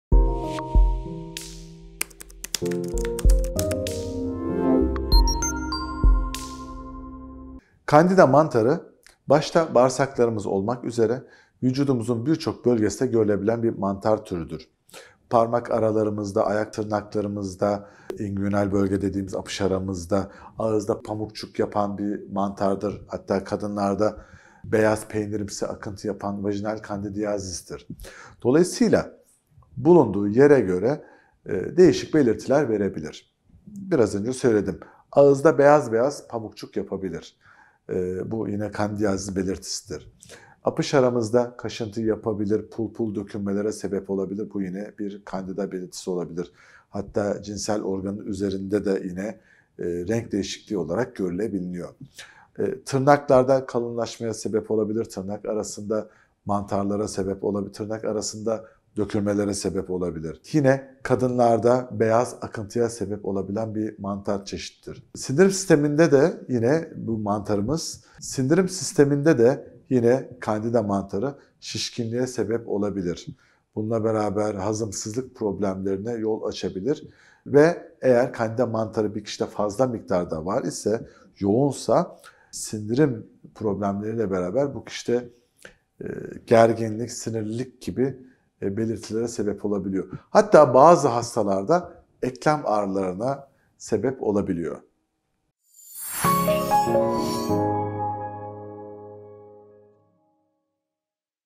Candida mantarı ne gibi semptomlara yol açar?
Candida mantarı başta bağırsaklarımız olmak üzere vücudumuzun birçok bölgesinde görülebilen bir mantar türüdür. Parmak aralarımızda, apış aralarımızda, ağızda... Ağızda pamukçuk yapar, hatta kadınlarda beyaz peynirimsi akıntı yapan vajinal kandidiyazistir. Bulunduğu yere göre değişik belirtiler verebilir. Apış aramızda kaşıntı yapabilir. Pul pul dökülmelere sebep olabilir. Cinsel organın üzerinde de renk değişikliği olarak da görülebiliyor. Candida mantarı. Tırnaklarda kalınlaşmaya sebep olabilir, tırnak arasında mantarlara sebep olabilir, tırnak arasında dökülmelere sebep olabilir. Kadınlarda beyaz akıntıya sebep olabilir. Sinir sisteminde de yine bu mantarımız sindirim sisteminde de yine Candida mantarı şişkinliğe sebep olabilir. Hazımsızlık problemlerine yol açabilir ve eğer Candida mantarı bir kişide yoğun miktarda var ise sindirim problemleri ile birlikte bu kişide gerginlik, sinirlilik gibi belirtilere sebep olabilir. Hatta bazı hastalarda eklem ağrılarına sebep olabiliyor.